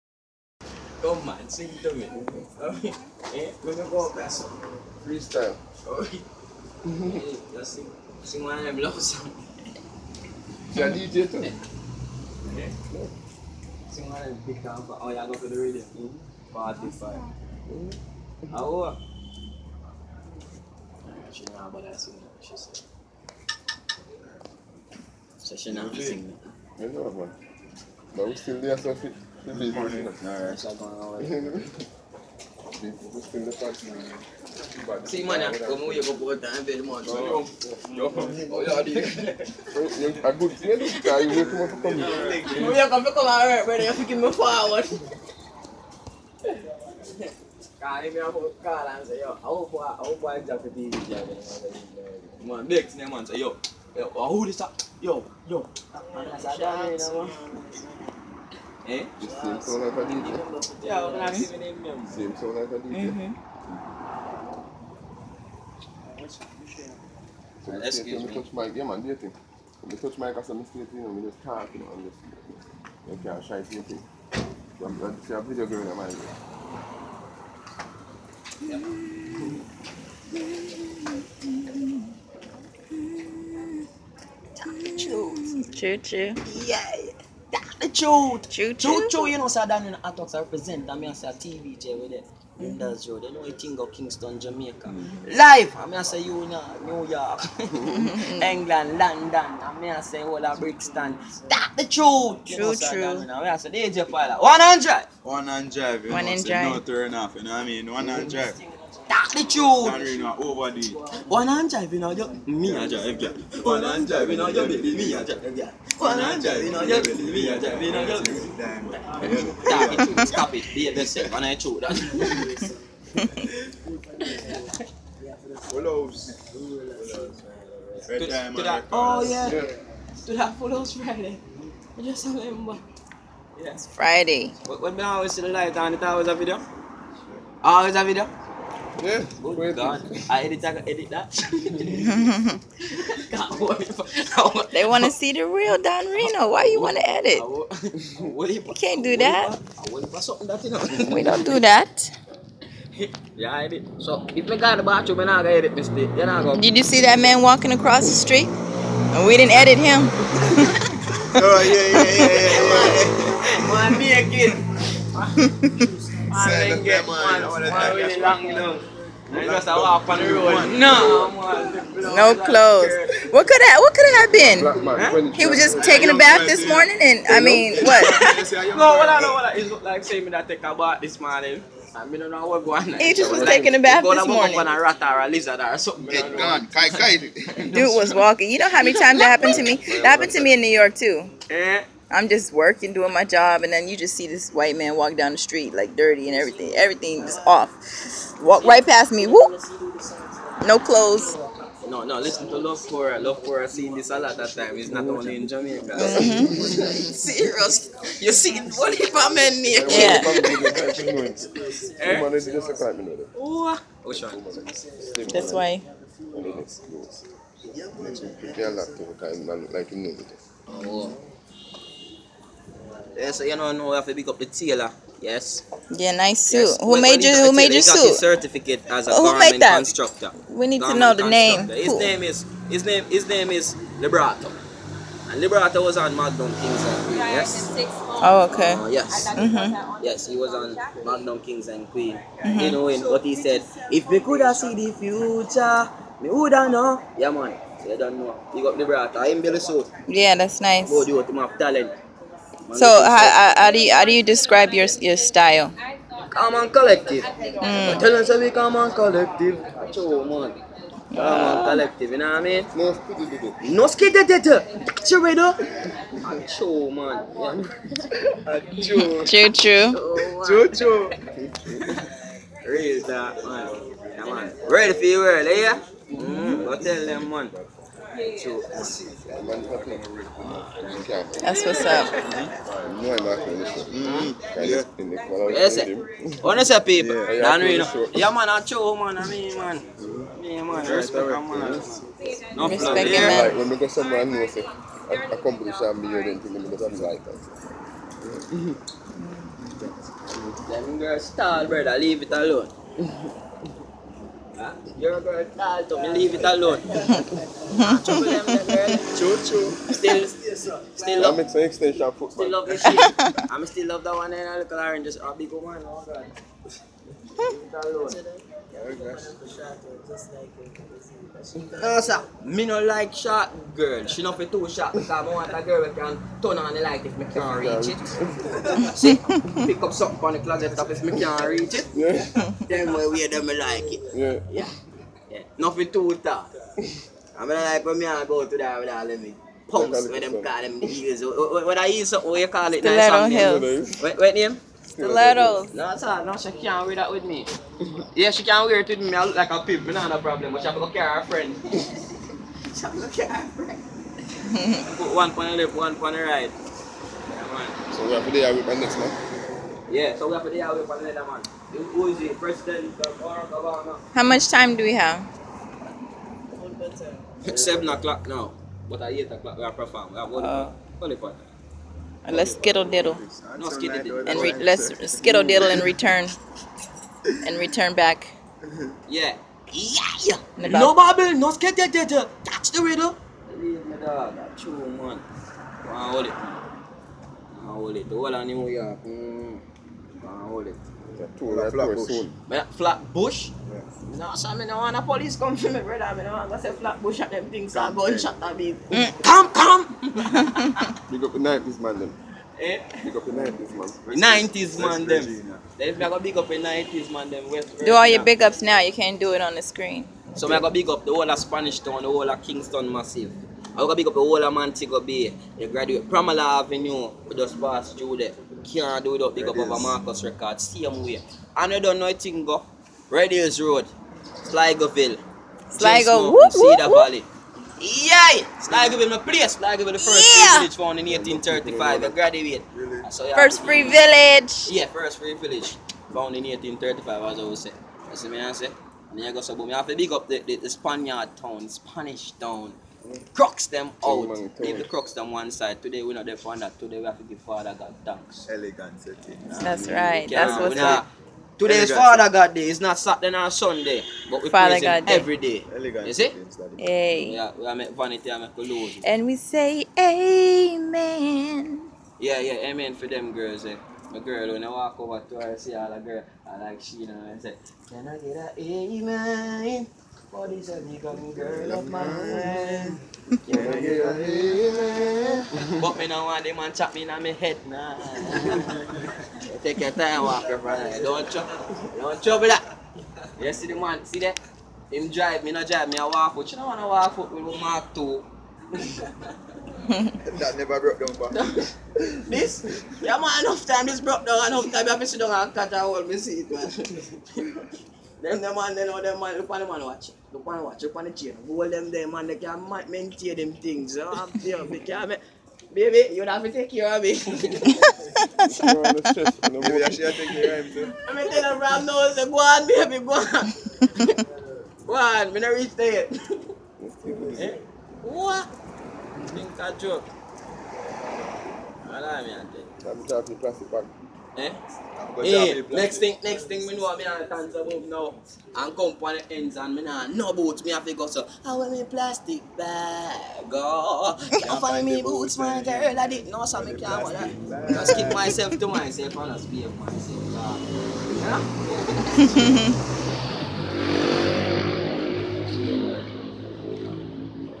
Dan Reno Behind da scenes TVJ Nuhskiddlediddle
Dan Reno and Red Diamond Entertainment Management Babylove, I-Will and Dj Pilot chat with TVJ staff member Likkle Peppa at TVJ Parking lot before Dan Reno performance Kingston, Jamaica!